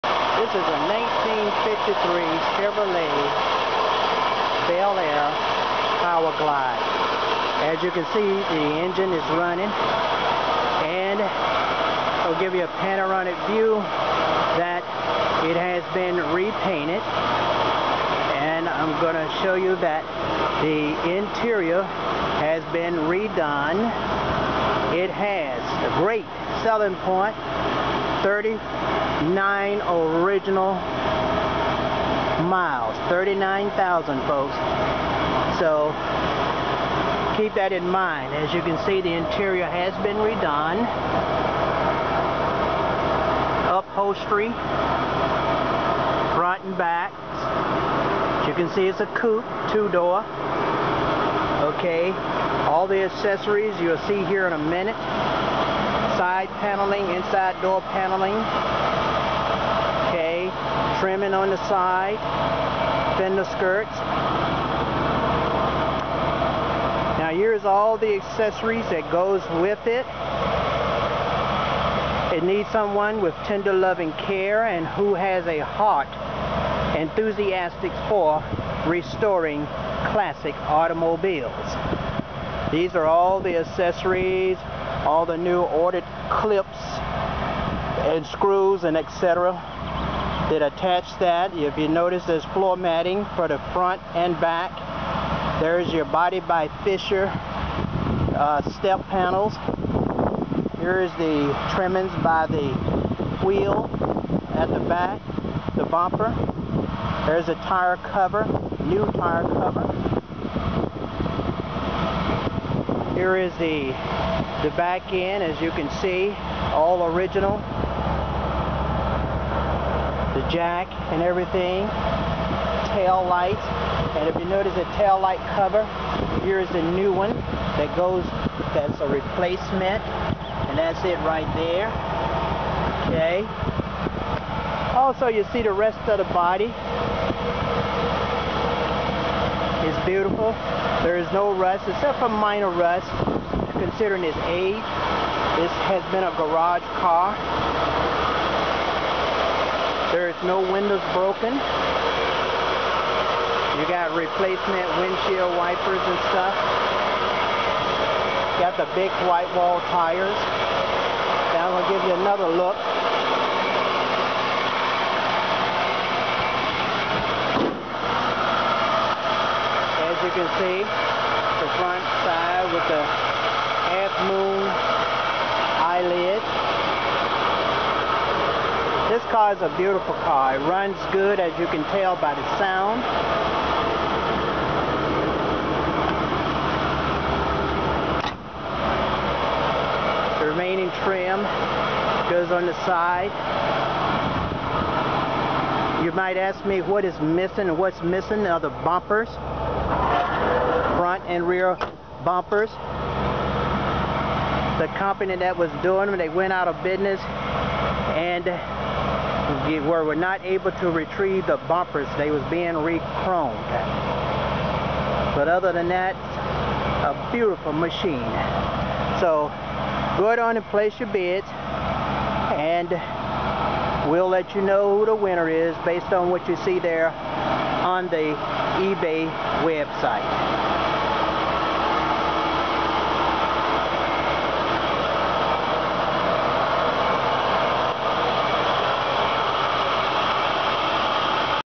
1953 Chevrolet Bel Air Video
This is a great car for the person that loves old cars.  It is being "as is".  The video and pictures on eBay says it all.